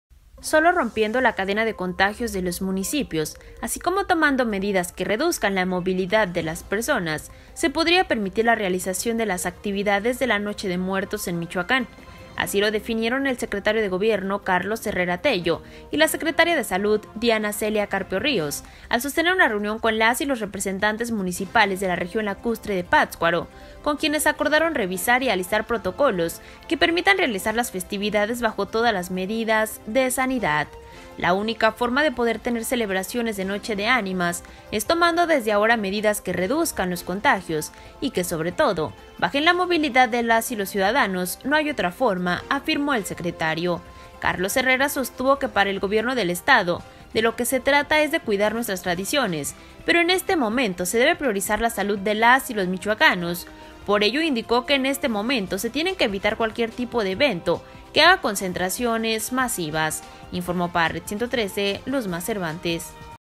Noche de Muertos, condicionada a la reducción de la cadena de contagios
El secretario de Gobierno, Carlos Herrera Tello; la secretaría de Salud, Diana Carpio Ríos, y la titular de Turismo, Claudia Chávez López, sostuvieron una reunión con los alcaldes de la región Lacustre de Pátzcuaro

Morelia, Michoacán, a 25 de agosto de 2020.- Solo rompiendo la cadena de contagios de los municipios, así como tomando medidas que reduzcan la movilidad de las personas, se podría permitir la realización de las actividades de la Noche de Muertos en Michoacán.

Así lo definieron el secretario de Gobierno, Carlos Herrera Tello y la secretaria de Salud, Diana Celia Carpio Ríos, al sostener una reunión con las y los presidentes municipales de la región Lacustre de Pátzcuaro, con quienes acordaron revisar y alistar protocolos que permitan realizar las festividades bajo todas las medidas de sanidad.

“La única forma de poder tener celebraciones de Noche de Ánimas es tomando desde ahora medidas que reduzcan los contagios y que, sobre todo, bajen la movilidad de las y los ciudadanos, no hay otra forma”, afirmó el secretario.

Carlos Herrera sostuvo que para el Gobierno del Estado de lo que se trata es de cuidar nuestras tradiciones, pero en este momento se debe priorizar la salud de las y los michoacanos, por ello indicó que en este momento se tienen que evitar cualquier tipo de evento que haga concentraciones masivas.

“Coincido con ustedes, tenemos que tomar acciones concretas, porque si no la ciudadanía no hace caso, quiero solicitarles que no inviten a que la gente se junte, y nosotros como Gobierno estatal los vamos a respaldar, de momento no hagan desfiles, Grito, ningún evento que invite a la aglomeración”, externó.

Detalló que contarán con el respaldo de la Guardia Nacional a fin de poder realizar acciones de dispersión, así como con Guardianes de la Salud para supervisar que los establecimientos cumplan con todos los requerimientos de sanidad.

Por su parte la secretaria de Salud, Diana Celia Carpio, expresó que también es necesario atender Morelia, Uruapan y Pátzcuaro como municipios con una alta movilidad de ciudadanas y ciudadanos hacia las zonas donde se realizan las festividades de la Noche de Muertos.

En ese sentido manifestó que septiembre será un mes clave para reducir el número de contagios, por ello la necesidad de evitar cualquier situación, evento o festividad religiosa que aglutine a las personas.

La secretaria de Turismo, Claudia Chávez López, presentó medidas exactas que tendrán que tomar los municipios para que puedan llevar a cabo las celebraciones bajo protocolos estrictos de sanidad.

La funcionaria estatal aseveró que ante la epidemia se vislumbran festejos poco concurridos y, sobre todo, fuertes pérdidas económicas en el sector, sin embargo dijo que la prioridad es atender las recomendaciones del área de la salud.

En la reunión las y los presidentes municipales externaron diversas inquietudes y se dijeron dispuestos a trabajar de manera coordinada con las autoridades estatales a fin de buscar mecanismos que garanticen la integridad de las y los ciudadanos, además de buscar el cómo lograr llevar a cabo la Noche de Muertos bajo todas las medidas de sanidad.

En la reunión estuvieron presentes los alcaldes de Pátzcuaro, Víctor Manuel Báez Ceja; de Lagunillas, Macarena Chávez Flores; de Erongarícuaro, Adrián Melgoza Novoa; de Coeneo, Ariel Trujillo Córdova; de Quiroga, Alma Rosa Vargas Diazbarriga; y de Salvador Escalante, Jesús Lucas Ángel.

También estuvieron el presidente Alejandro García Serna, alcalde de Huiramba; Rolando Méndez Baldovinos, representante del edil de Tzintzuntzan; además Julio Fajardo, integrante de la Arquidiócesis de Morelia.